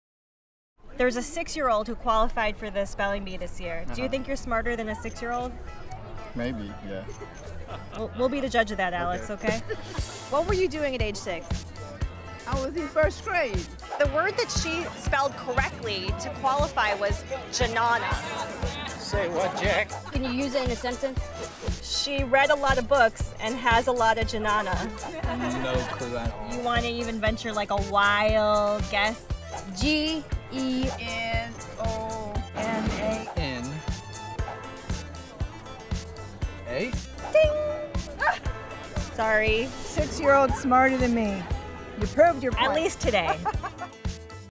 Are You Smarter Than a Six-Year-Old?
This year, six-year-old Edith Fuller became the youngest to ever qualify for the Scripps National Spelling Bee. Could you spell her winning word?